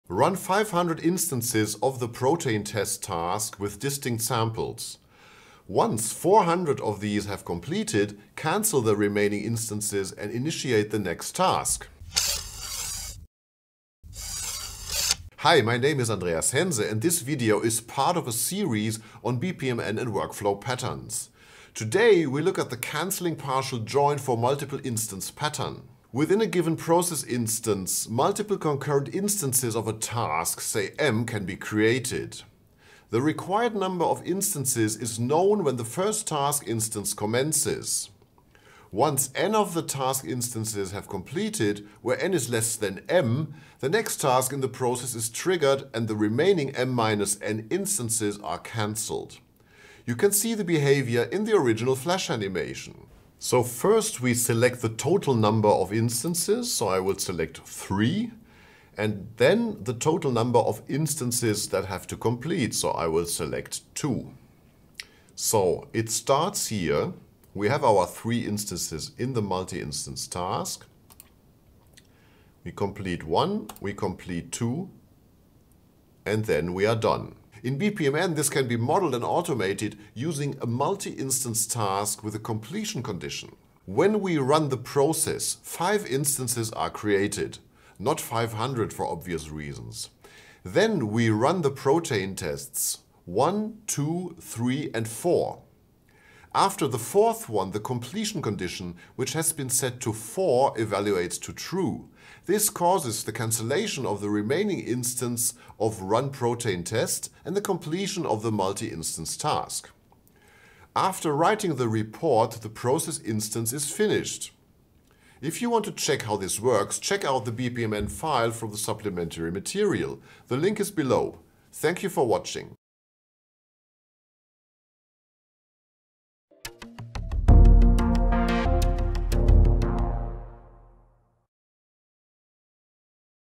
Cancelling Partial Join for Multiple Instances in BPMN
Workflow Optimization: Canceling Redundant Instances. This video illustrates a BPMN workflow pattern involving the execution of 500 instances of a protein test task. Once 400 of these tests are completed, the remaining instances are canceled, initiating the next task.

This video is part of a series on modeling the workflow patterns with BPMN.

This is Video Number 35 in the series on BPMN. You can find supplementary material on github:
Click on "Code"/Download zip to get the whole package. The material for this tutorial is in the folder that corresponds to the video number.

00:00 Introduction
00:21 Cancelling Partial Join for Multiple Instances Pattern
01:17 Cancelling Partial Join for Multiple Instances in BPMN
01:54 Conclusion